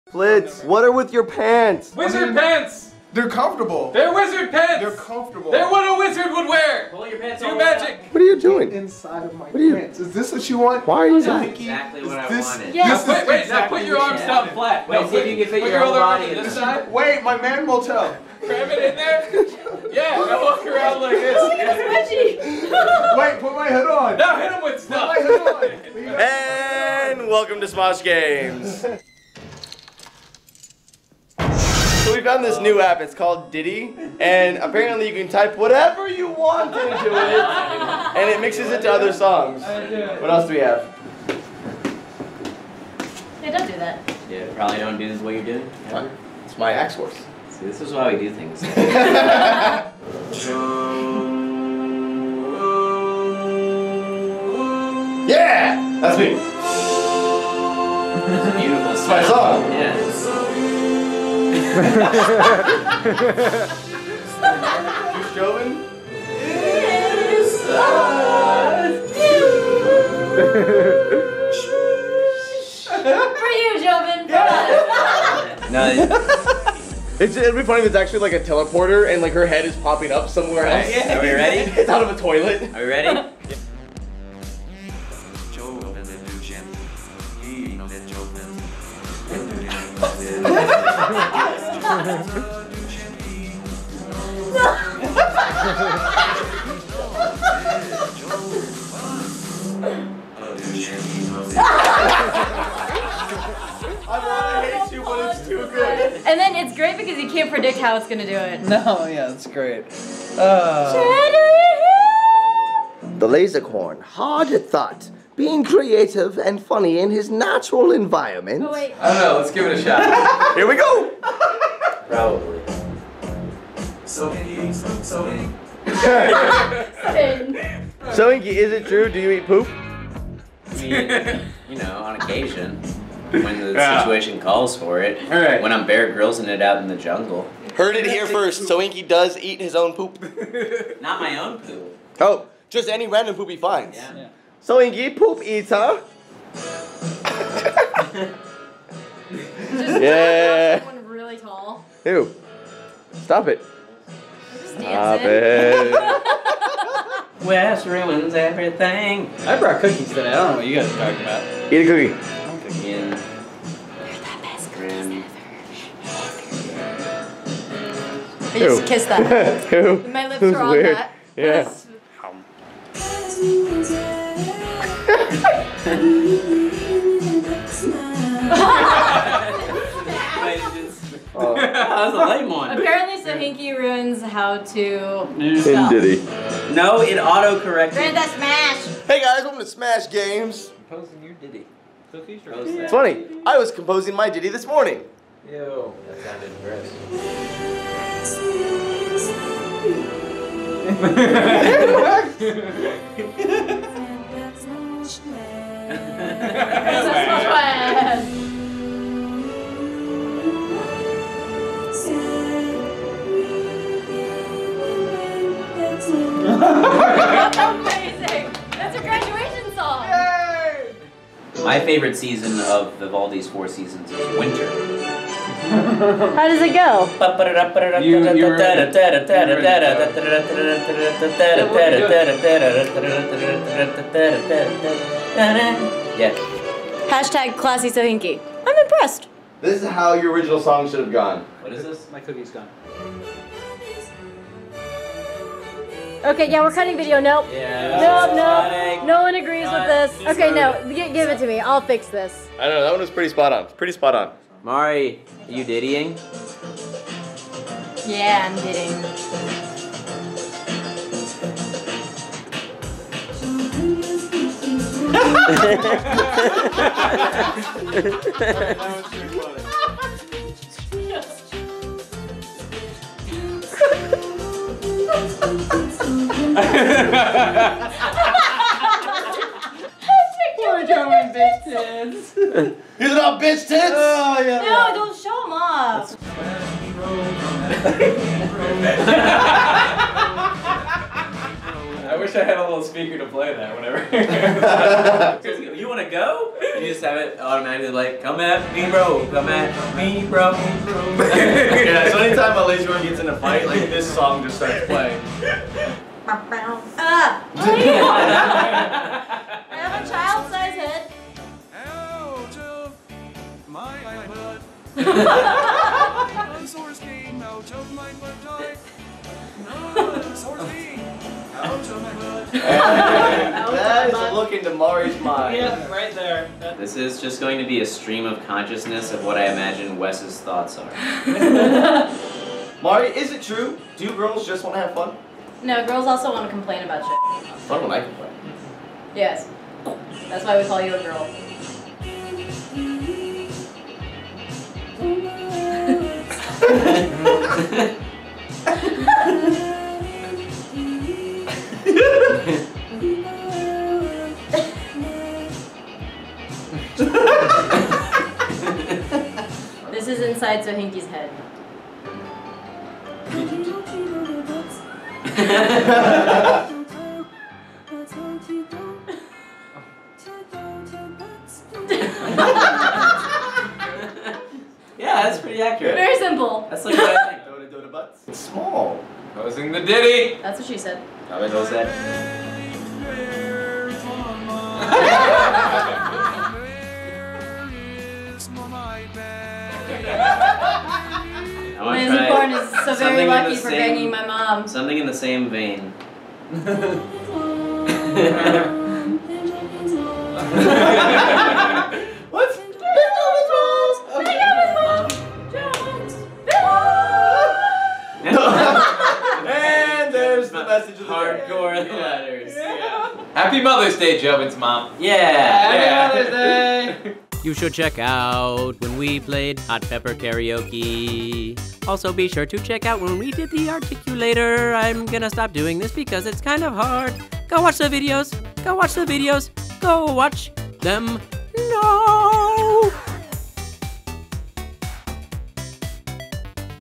FUNNIEST SONGS EVER (Bonus)
Ditty is a really fun app that let's you turn any words you can thing of into fun pop songs! It's especially fun when playing with a bunch of friends who like busting each others balls!